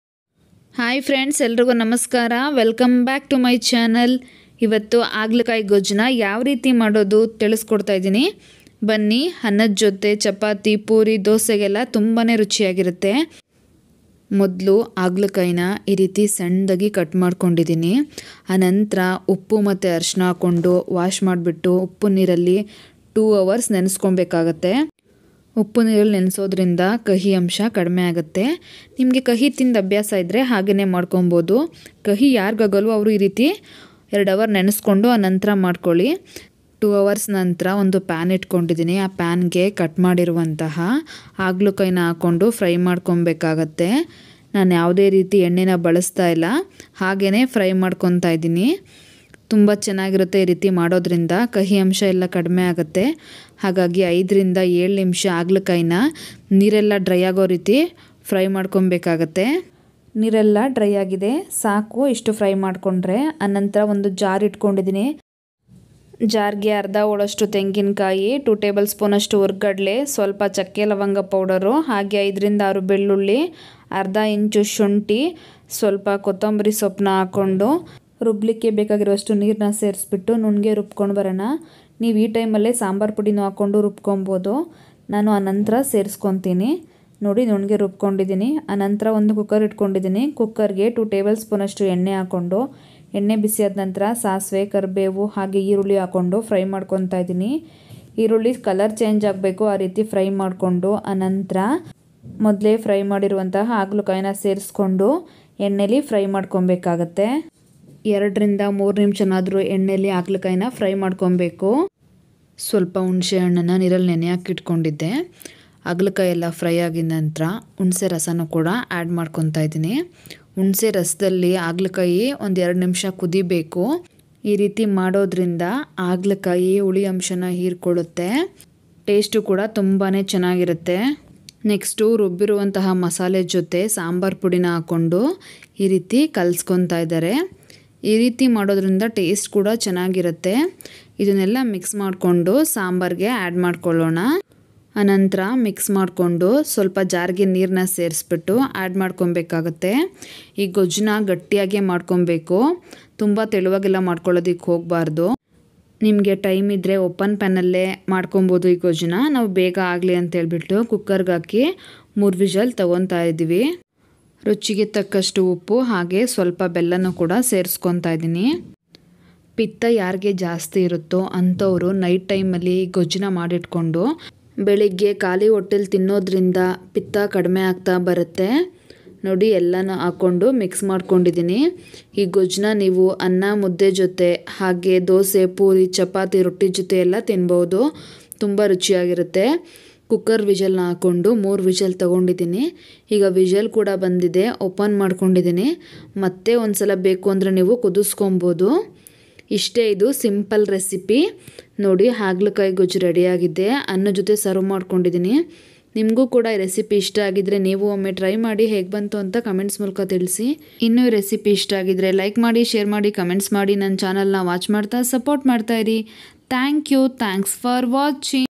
ಸುಲಭವಾಗಿ ಕಹಿಯಿಲ್ಲದ ಹಾಗೆ ಹಾಗಲಕಾಯಿ ಗೊಜ್ಜು ಮಾಡುವ ವಿಧಾನ/bitter gourd gravy recipe/Hagalakayi gojju
ಸುಲಭವಾಗಿ ಕಹಿಯಿಲ್ಲದ ಹಾಗೆ ಹಾಗಲಕಾಯಿ ಗೊಜ್ಜು ಮಾಡುವ ವಿಧಾನ/ bitter gourd gravy recipe in kannada/ Hagalakayi gojju

Ingredients

bitter gourd
oil
mustard seeds
curry leaves
onion
coconut
fried gram
cinnammon
cloves
ginger
garlic
sambar powder
water
jaggery
salt
tamarind juice